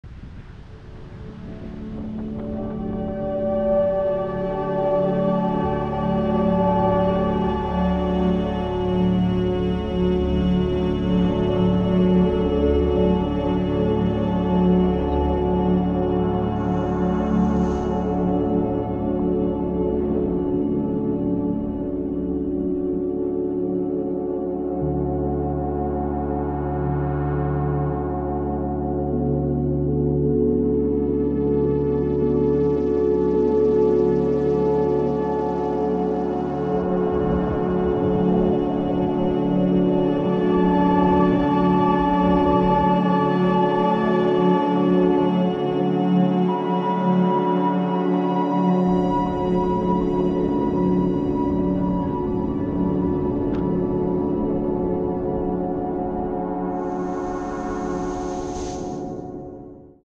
Surfebruary Ep 6
Chilled Sunday and bodysurf vibes for SurFebruary
raising funds for cancer research.